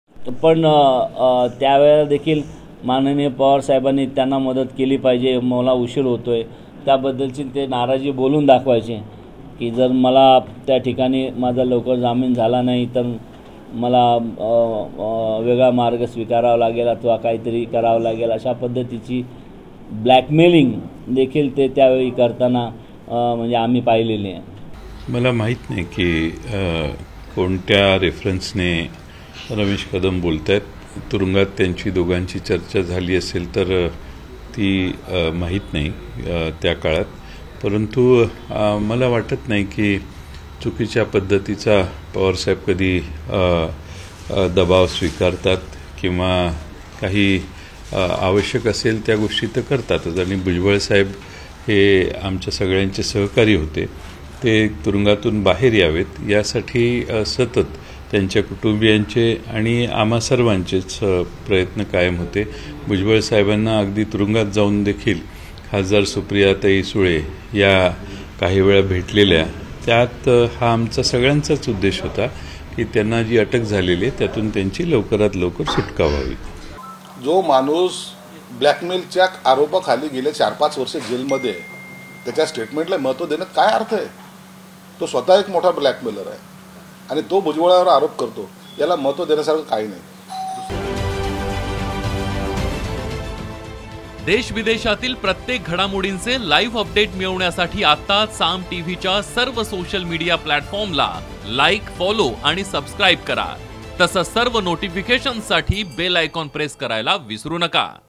Ramesh Shinde Vs Jayant Patil News | राष्ट्रवादीच्या दोन्ही गटात राजकारण तापलं
Ramesh Shinde Vs Jayant Patil News | मंत्री छगन भुजबळ कारागृहात असताना शरद पवारांना ब्लॅकमेल करायचे, असा खळबळजनक आरोप राष्ट्रवादी काँग्रेसचे नेते रमेश कदम यांनी केला. त्यानंतर राष्ट्रवादीच्या दोन्ही गटातील राजकारण चांगलचं तापलं आहे. शरद पवार गटाचे प्रदेशाध्यक्ष जयंत पाटील यांनी रमेश कदम कशाच्या आधारे आरोप करतात, माहित नसल्याचं स्पष्ट केलं आहे. त्यामुळे याप्रकरणी आता राजकारण रंगत असून आरोपप्रत्यारोप होत आहेत.

Location : Mumbai
Video Produced By : Soni Magar Sonipmagar
Video Edited By : Riddhesh Tare
Reporter : Saam Team Input